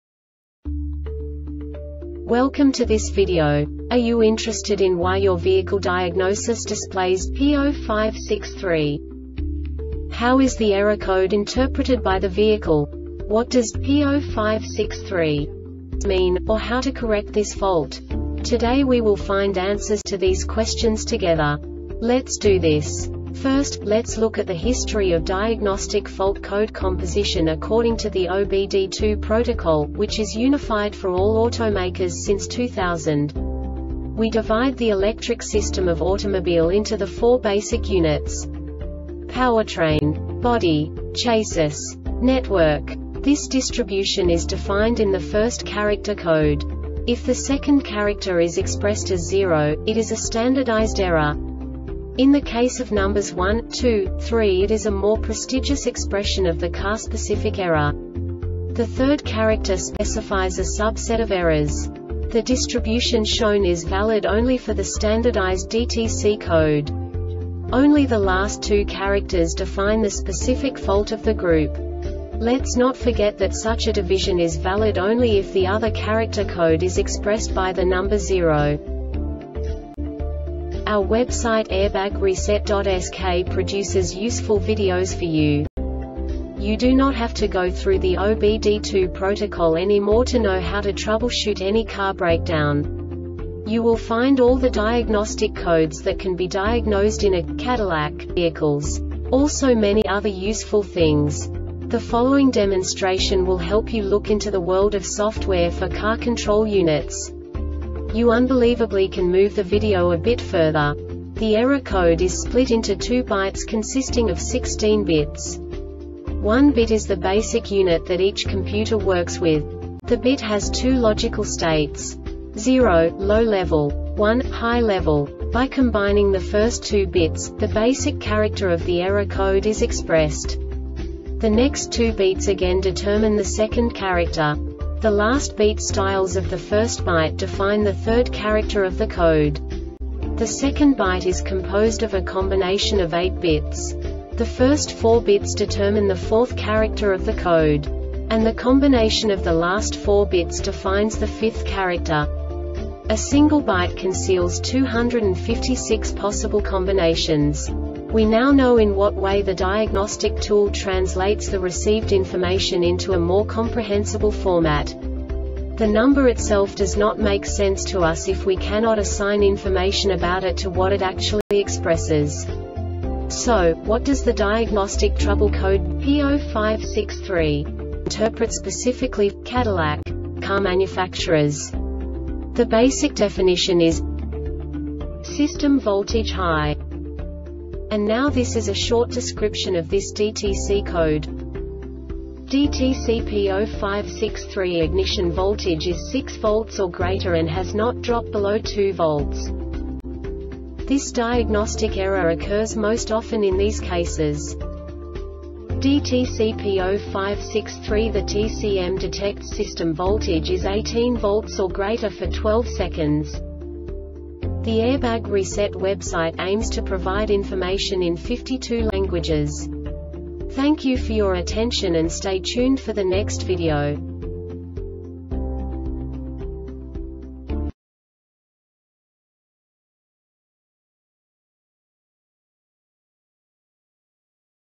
DTC cadillac P0563 Short Explanation
Contents:
0:21 Basic DTC analysis according to OBD2 protocol standard.
1:48 Insight into programming
2:58 A diagnostically understandable description of the Diagnostic Trouble Code
3:05 Basic error definition

Make:
Cadillac
Code:
P0563
Definition:
System Voltage High
Description:
DTC P0563 Ignition voltage is 6 volts or greater and has not drop below 2 volts.
Cause:
DTC P0563
The TCM detects system voltage is 18 volts or greater for 12 seconds.